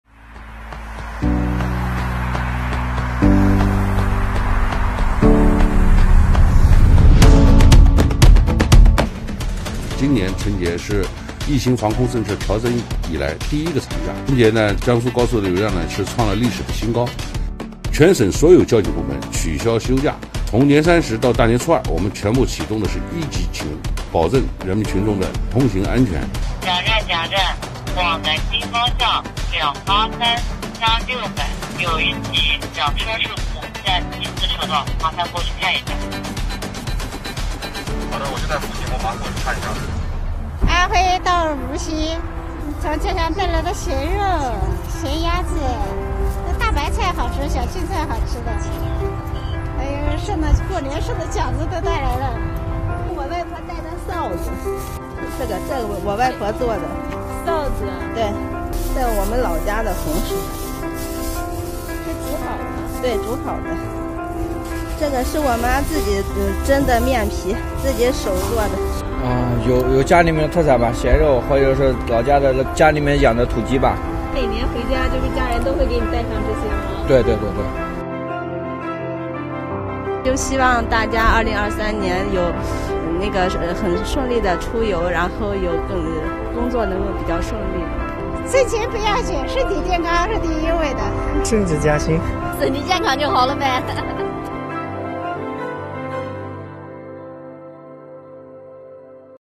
What's in your car boot on the way back from home after Chinese New Year?
During the past SpringFestival, expressway traffic volume in Jiangsu hit a historic high. On people’s way back from home, they have the car boot fully loaded with luggage, and also love and hope for a new year.